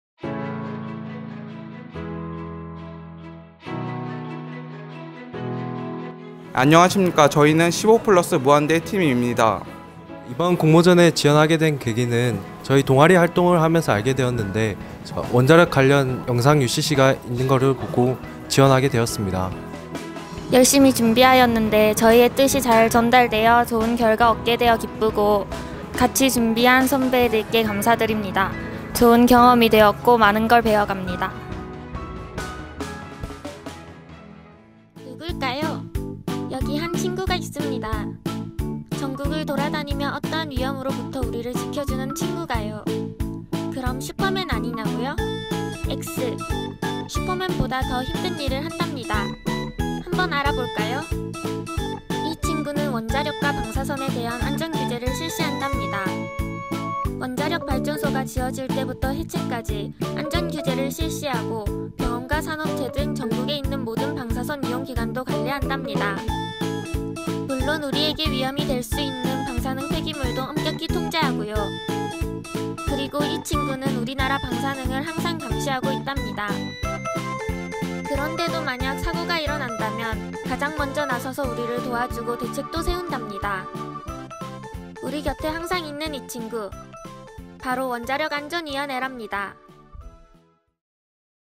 원자력안전위원회, UCC로 표현해주세요!
원자력안전위원회 표현하기 UCC&슬로건 공모전에서는 원자력안전위원회를 어떻게 표현했을까요? 우수상을 수상한 UCC를 통해 살펴보세요!